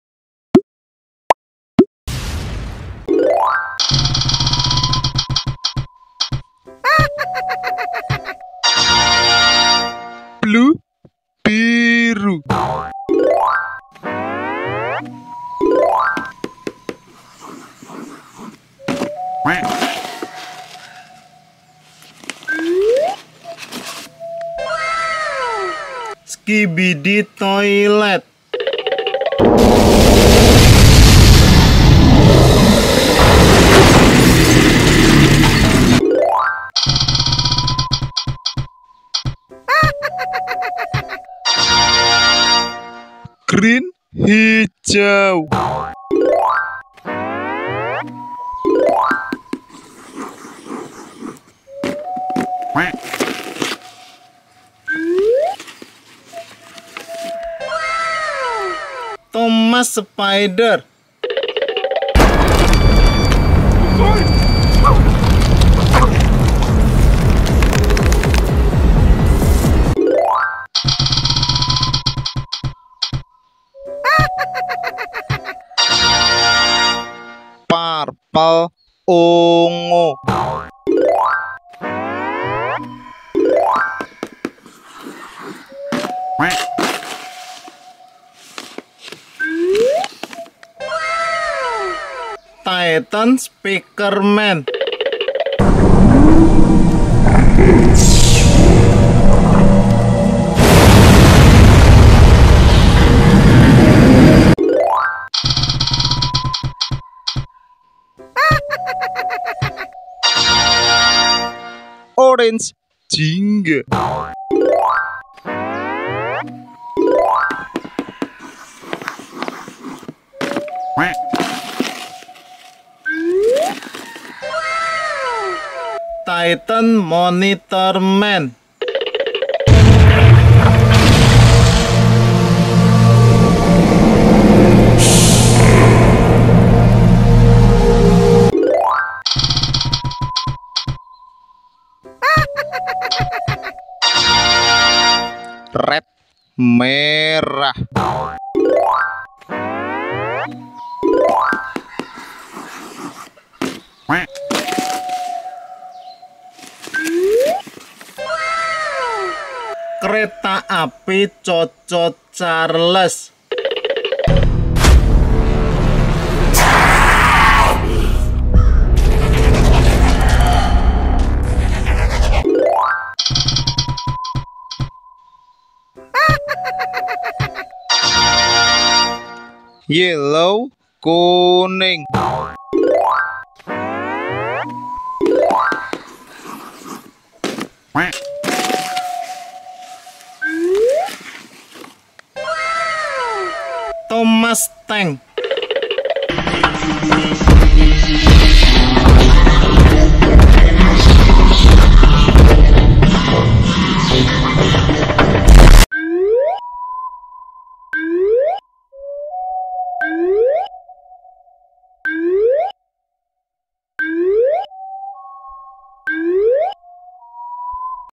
Spin Wheel Monster !! Thomas Spider, Choo Choo Charles, Skibidi Toilet, Thomas Tank ...
Welcome to "Trucks with Trailer 01" Today we will provide education about the GODZILLA MONSTER, which is still one of the trending films, including ...

- Thomas Tank
- Choo Choo Charles
- Thomas Exe
- Titan Speaker Man
- Skibidi Toilet
- Titan Monitor Man

Hope you are entertained... THANK YOU !!